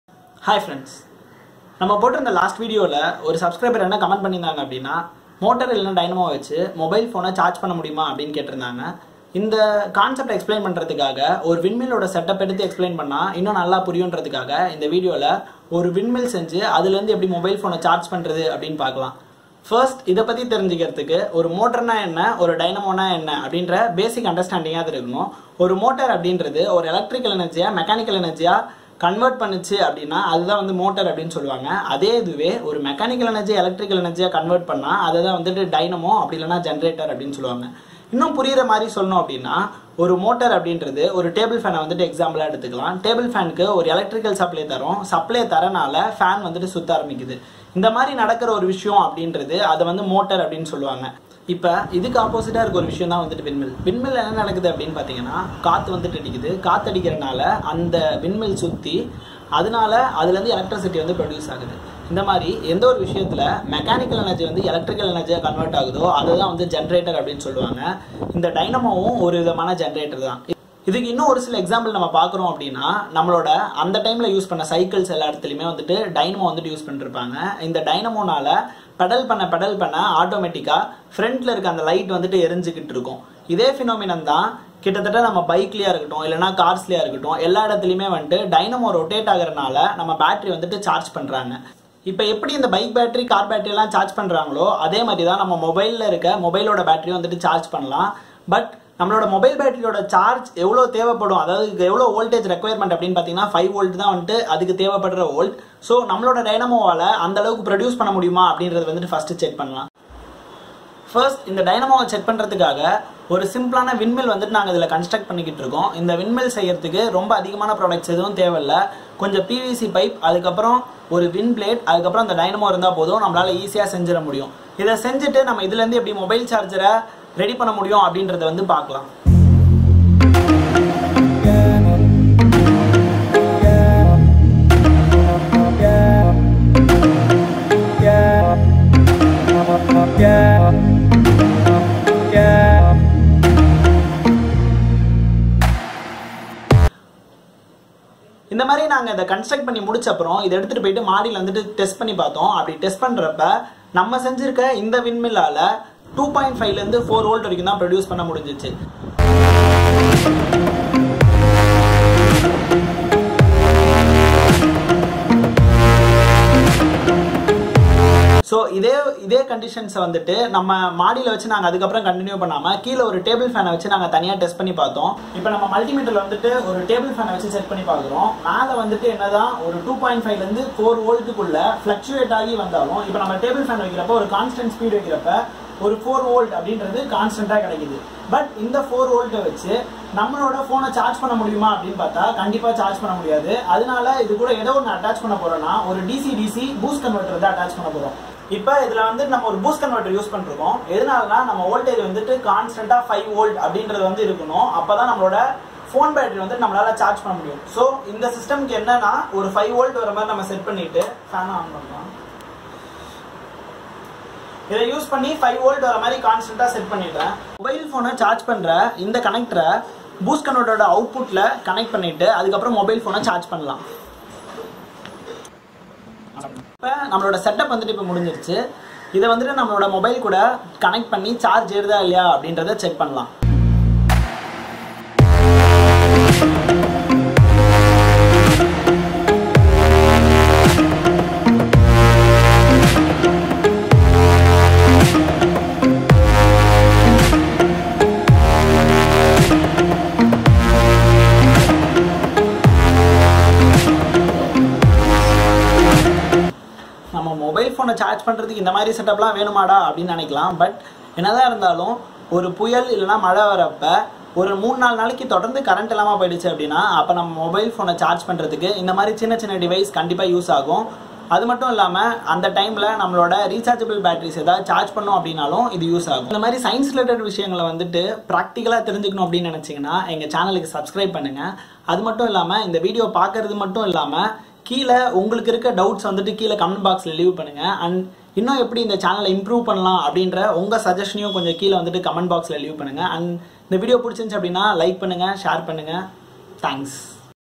WINDMILL MOBILE CHARGER|SIMPLEST WINDMILL|DIY|TAMIL
In this we explained the use and application of dynamo and also how to consruct a DIY windmill. By using this WINDMILL our MOBILE phones can be charged. However a boodt converter is need for our practical application.